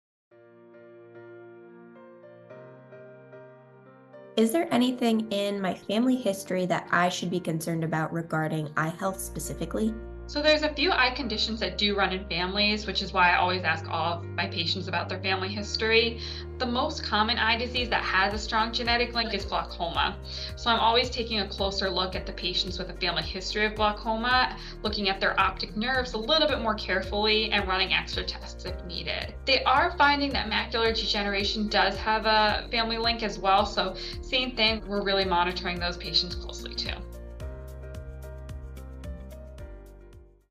Certain genetic eye conditions can affect your vision.
If you have a family history of glaucoma, macular degeneration, and retinitis pigmentosa, you must stay vigilant and schedule regular eye exams. Early detection and proactive care can help manage and preserve your eye health. Don't wait—take action today!